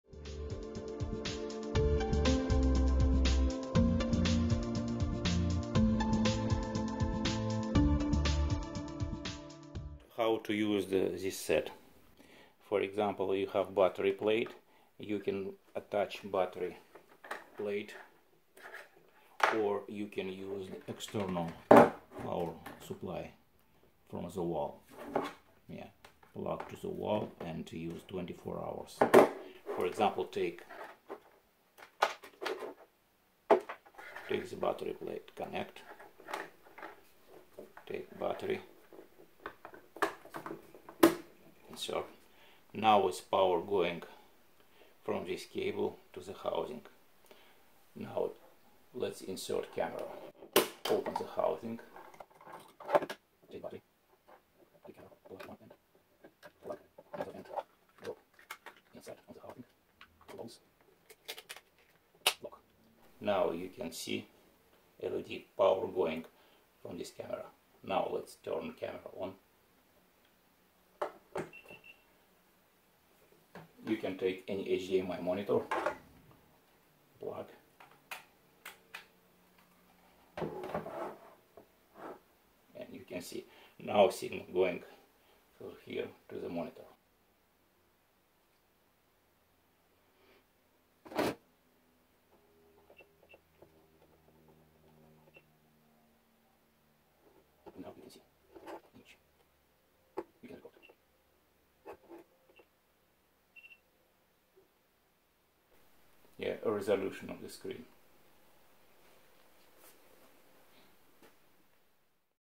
GoPro Live Stream underwater Hero 7 solutions with DNC-GP10 system
Underwater housing for GoPro Hero7/6/5 cameras with Live Video Stream 3840x2160 30Hz or 1080p60
You can stream Live Video from underwater to the surface, over 10 meters External HDMI and 10 meter Power cables.
4k HDMI signal produced over HDMI only Hero5.
Hero 7 maximum HDMI output resolution is 1080p60.
Power cable feeds the GoPro from the surface for extended usage.
For detailed information see Dive And See web site